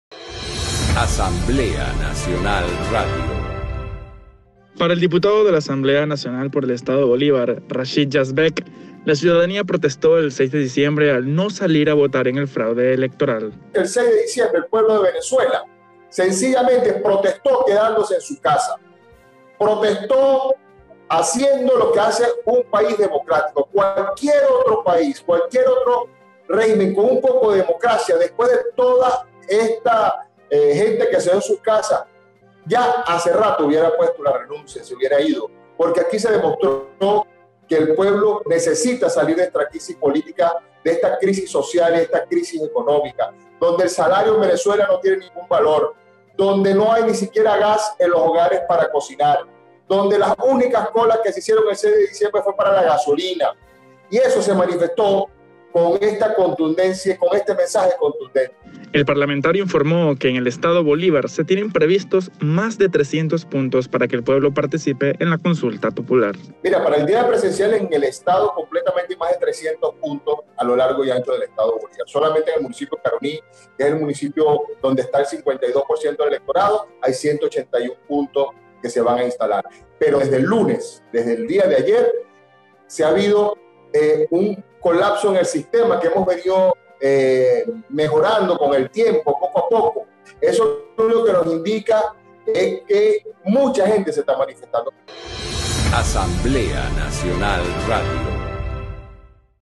Dip. Rachid Yasbek asegura que en Bolívar hay 300 puntos para la Consulta Popular (8.12.20)
"Venezuela protestó quedándose en su casa. En una democracia, después de estos resultados hubiera puesto la renuncia", así inició su intervención el dip. Rachid Yasbek.

También, informó que en Bolívar habrán más de 300 puntos de participación en la Consulta Popular.